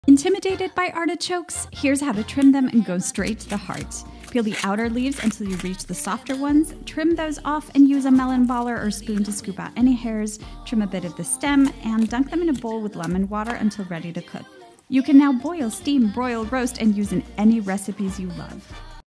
A quick tip and the easiest way to trim artichokes
A quick kitchen hack for when you quickly need to get to that artichoke heart to use in recipes.